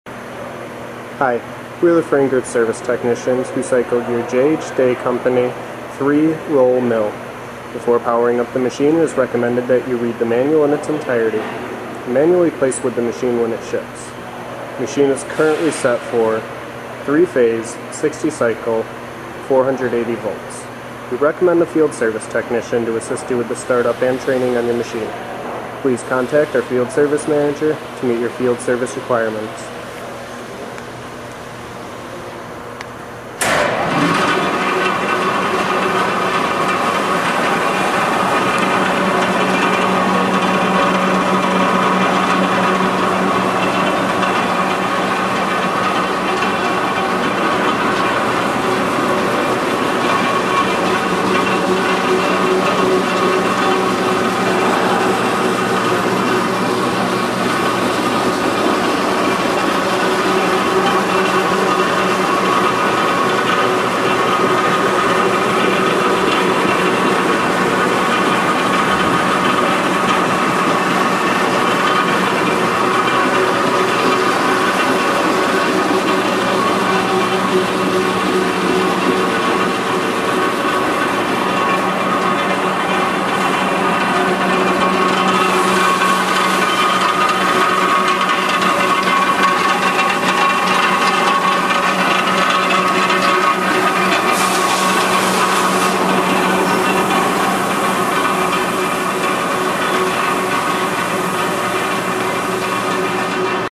J H Day 3 ROLL Mill Roller
To view this piece of machinery on our

3-Roll dispersion mill has (3) 14"diameter x 30"long rollers.  Unit complete with apron, end plates and a hydraulic roll set system.  Dispersion mill capacity of 700 lbs/hour (based on heavy paste mix [approx 14lbs/gal] using an average dispersion film thickness of .0005"). Primarily used to grind cosmetics, printing ink, lacquers, chocolates, plastics, ointment and paints.  Water cooled rolls made of hardened steel.